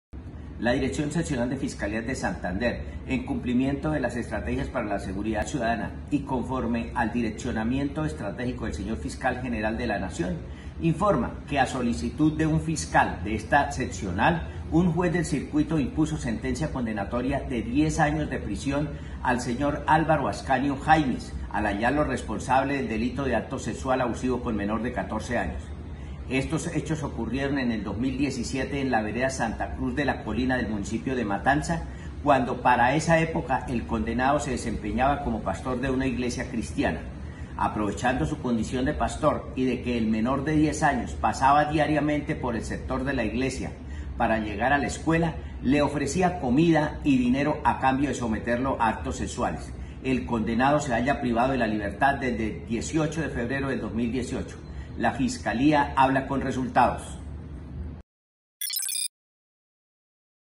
Condenado a 10 años de prisión expastor de iglesia cristiana por abuso sexual a una menor
Ante las pruebas presentadas por la Fiscalía, un juez de conocimiento condenó a 10 años de prisión a Álvaro Ascanio Jaimes, quien en 2017 —cuando se desempeñaba como pastor de una iglesia cristiana— abusó sexualmente de un menor de edad, en Matanza, Santander.